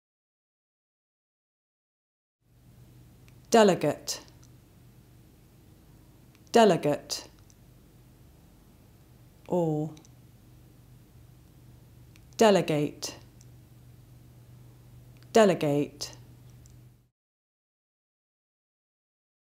How to pronounce DELEGATE in British English
This video shows you how to pronounce DELEGATE in British English. Speaker has a received pronunciation accent. Collins Dictionary, the home of living English and pioneers of dictionary publishing